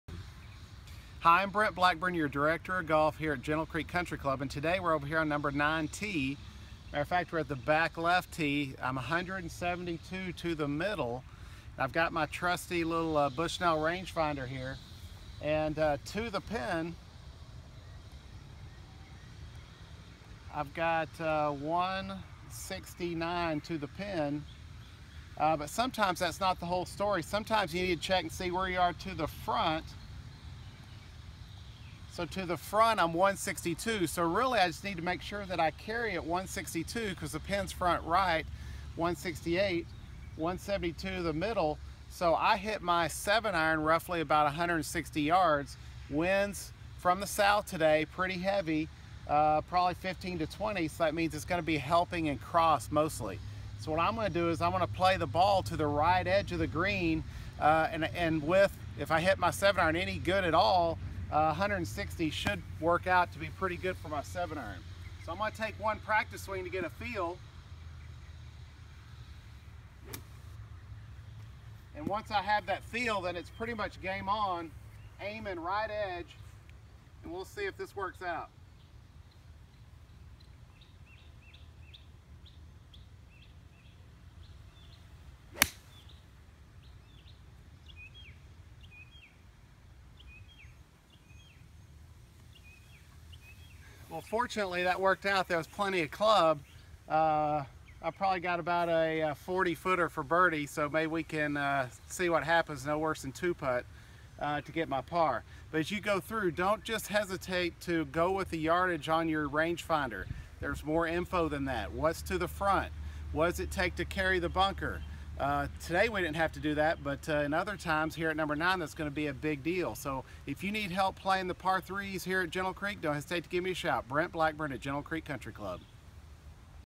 Ask the Pro with Brent Blackburn, playing #9 a par 3
Make sure you take enough club to get to the par 3's.